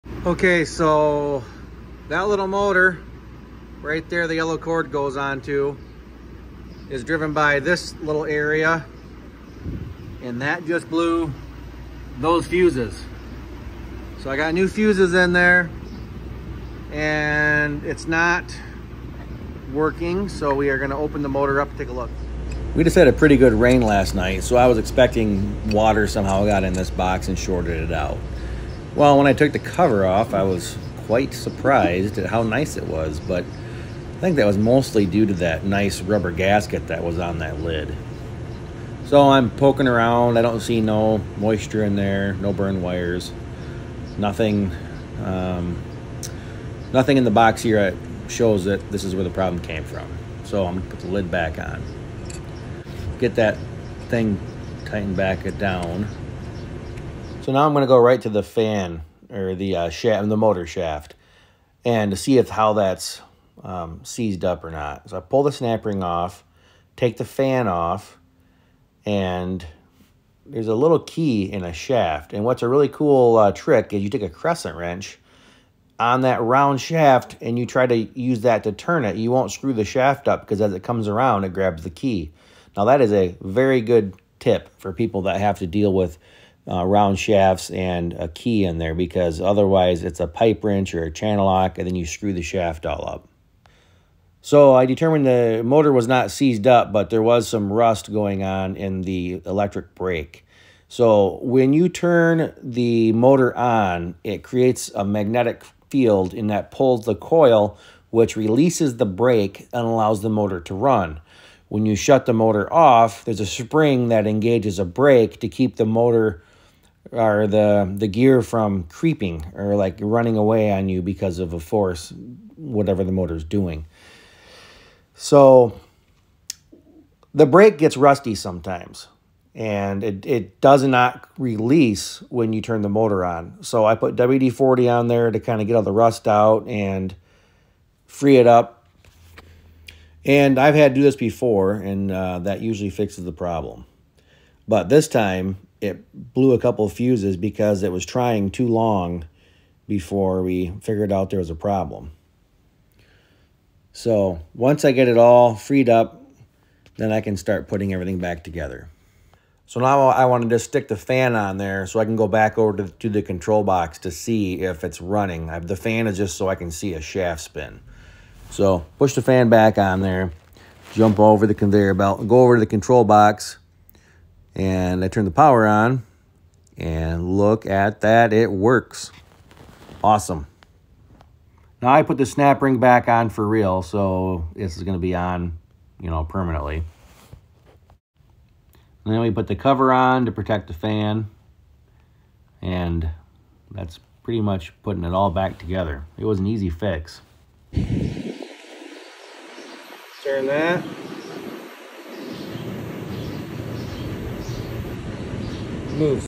Fixing a break motor with WD-40
When a break motor is stuck sometimes you can free it up and make it work again. I just used some WD-40.